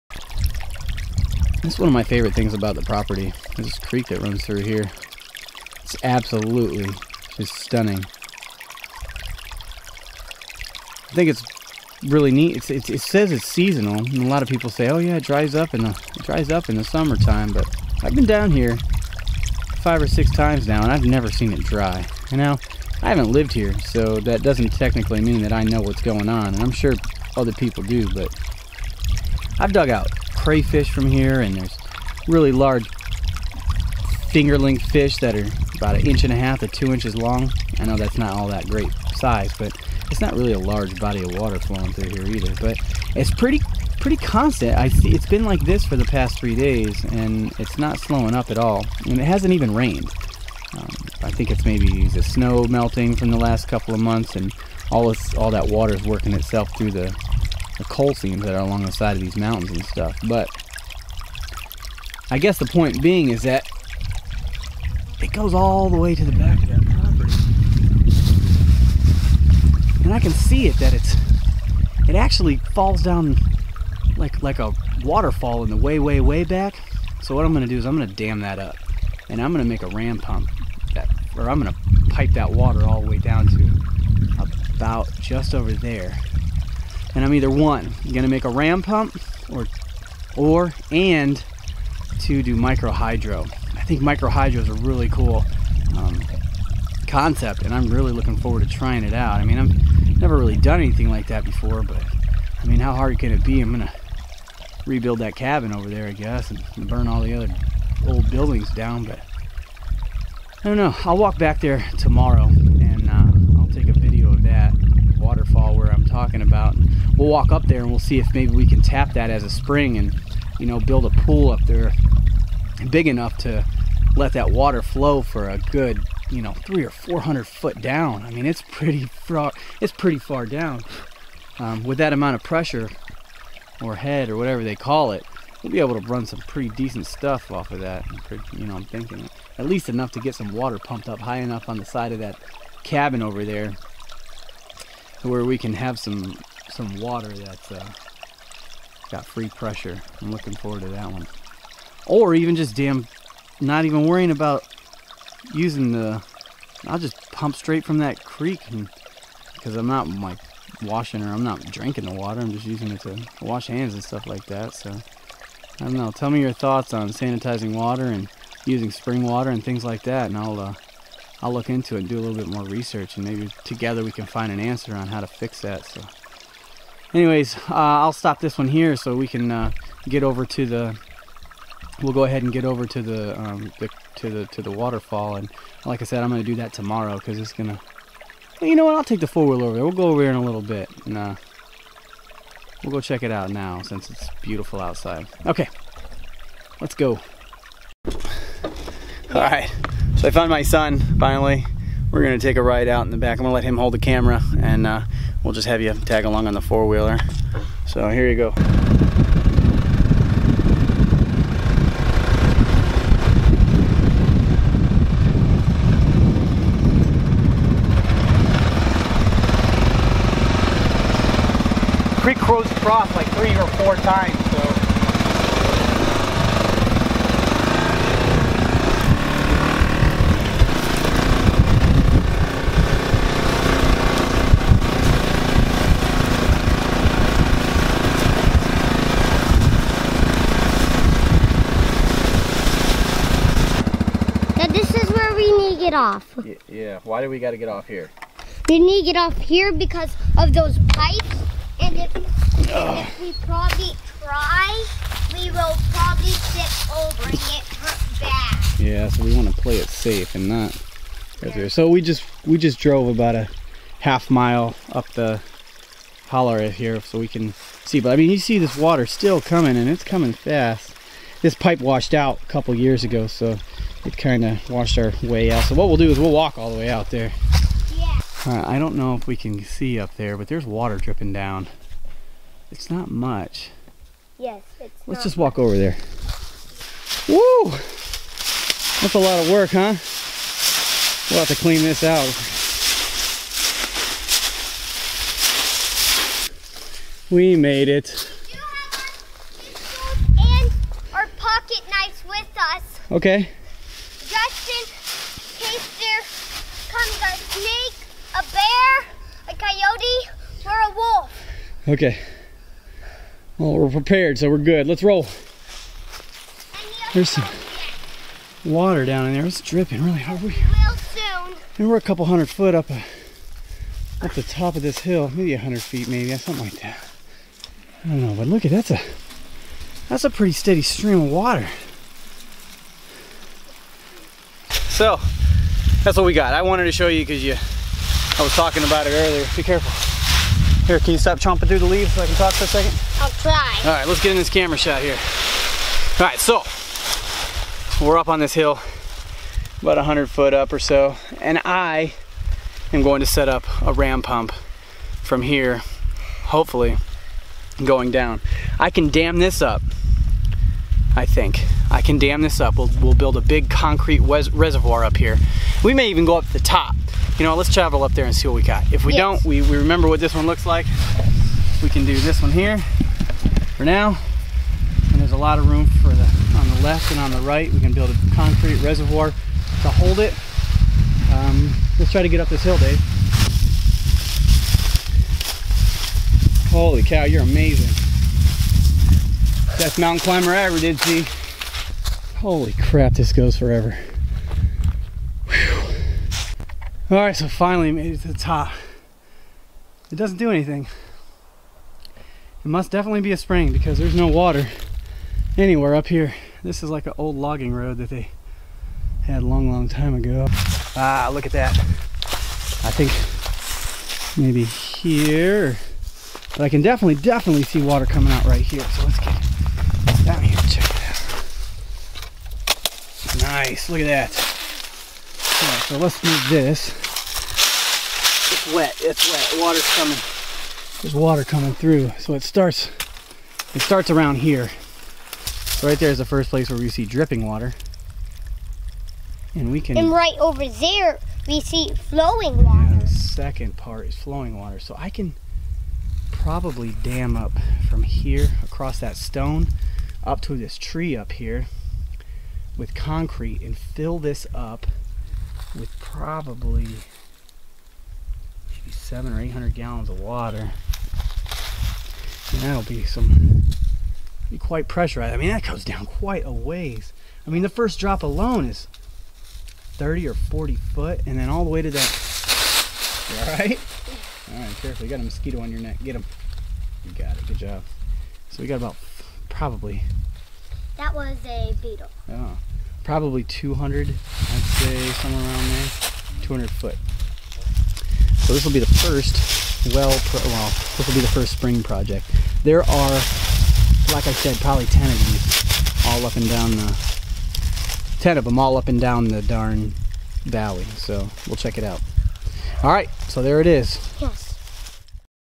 Off grid water solution, micro hydro, ram pump
Here is an intro to how I would like to start working on my off grid water storage, energy, etc.  I would like tips and tricks you have already tried and worked with so I am not reinventing the wheel.